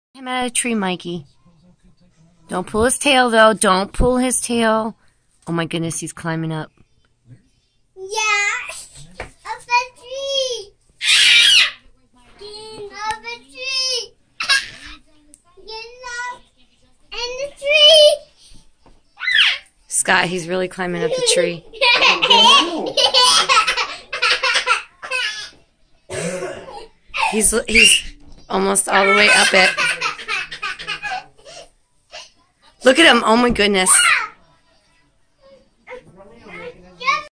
Ari Climbing the Christmas Tree
My cat is climbing the Christmas tree. This ought to be interesting this year!